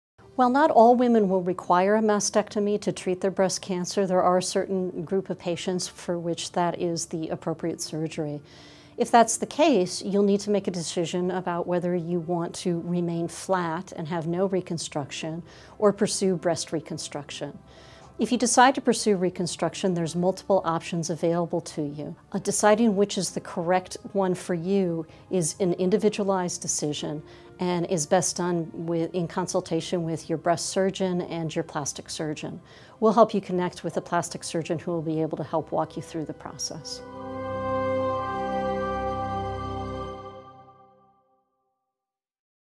Breast Reconstruction Options
Dr. Hendershott describes your options for breast surgery.